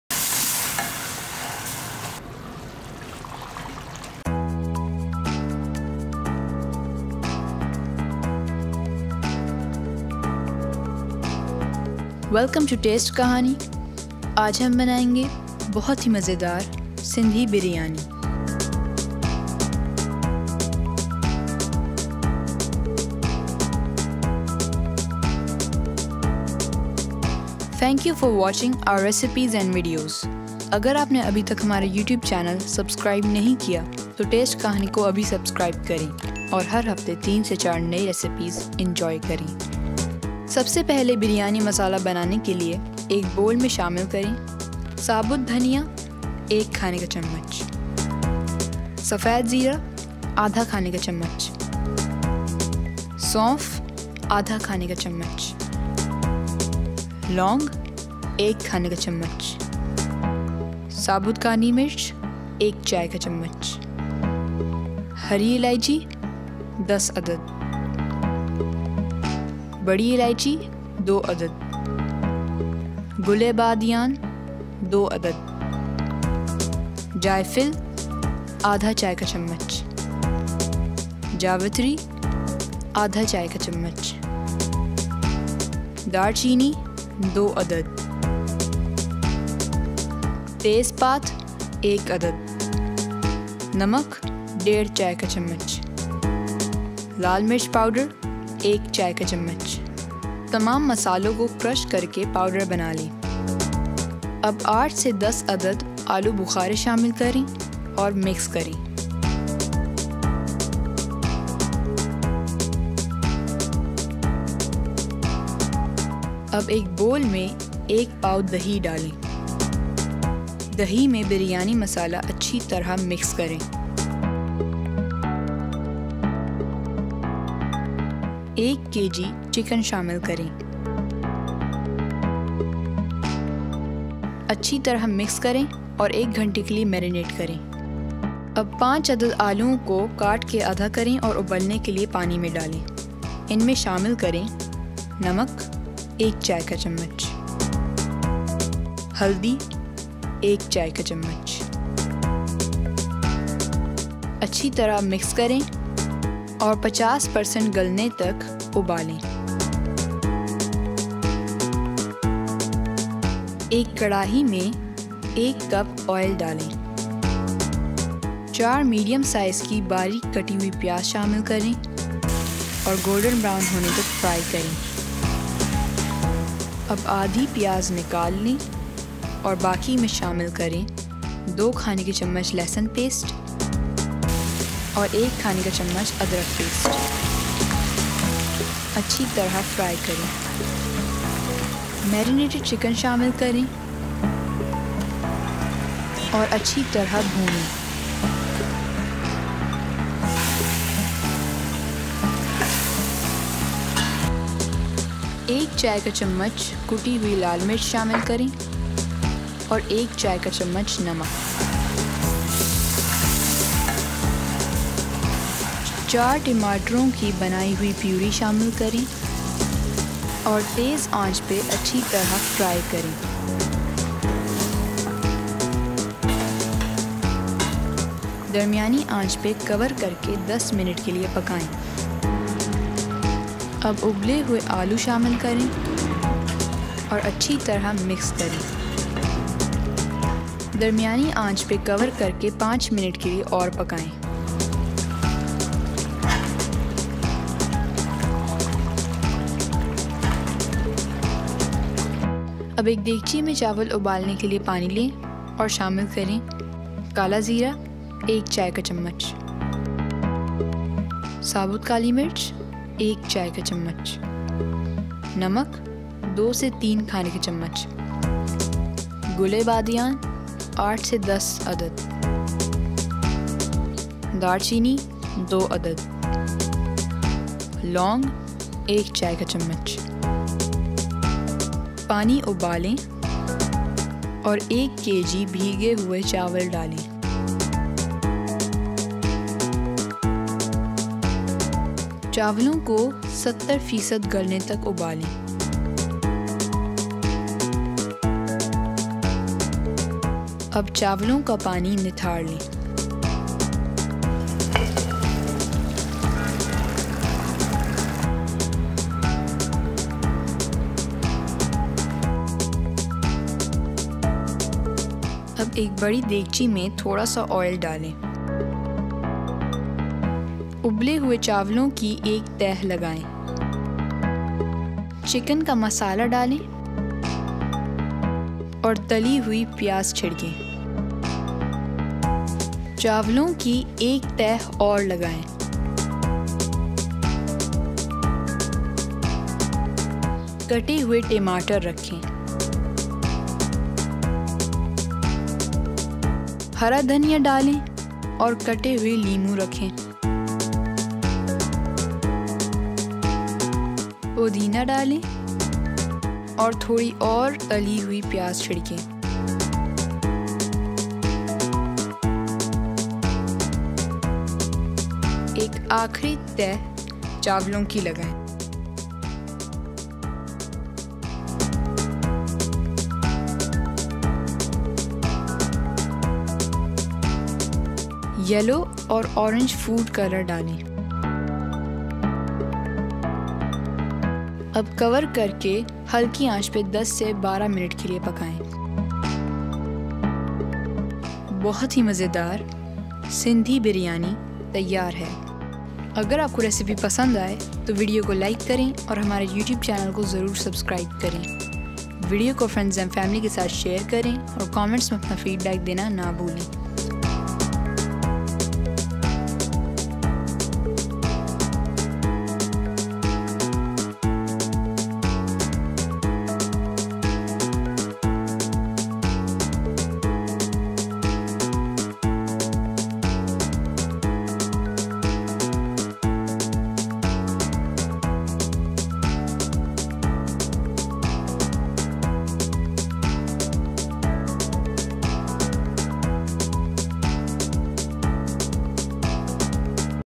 Sindhi Chicken biryani | سِندھی بِریانی |Ramazan Recipes | Ramadan | Sehri | Iftari
Sindhi Chicken biryani | Chicken Biryani | Sindhi Biryani | سِندھی بِریانی
Ramazan Recipes | Ramadan Recipes | Sehri | Iftari |

Step 1:     CHICKEN MARINATION:
                 Ingredients for Biryani Masala :
                Cloves  1 Tsp
                Black Pepper Corns   1 Tsp
                Nut Meg  1/2Tsp
                Mace   1/2 Tsp
Grind all the spices to make a Biryani Masala, add 8 to 10 Dried Plums, Mix with 1 Cup Yogurt.
Marinate 1 kg Chicken meat with the biryani masala for 1 Hour.

Step 2:    Boil Potatoes 5 ( cut in half) with 1 Tsp Salt and 1 Tsp Turmeric Powder. Boil till 50% done.

Step 3:    Making of Biryani Qorma:
Fry Onions till golden brown.    4 medium size ( finely sliced)
(Separate Half of the brown Onions for later use)
In the remaining add Crused Garlic 2 Tbsp,  Crushed Ginger 1 Tbsp, fry for a while then add Marinated Chicken, mix well and fry on medium to high flame till chicken turns white. Now add
Tomato Puree  ( 4 tomatoes blended)
Mix well and cook on high flame for a while then turn the flame to medium, cover and cook for further 10 minutes. Add boiled Potatoes and cook for further 5 minutes. Qorma is ready.

Step 4:    Boil Rice  1kg (Soaked & Drained) in 2 to 3 litre Water till 70% done along with:
                 Star Anise.  8 to 10
                 Salt. 2 to 3 Tbsp
                  Cloves.  1 Tsp
                  Cinnamon Sticks  2
                  Black Cumin Seeds.  1 Tsp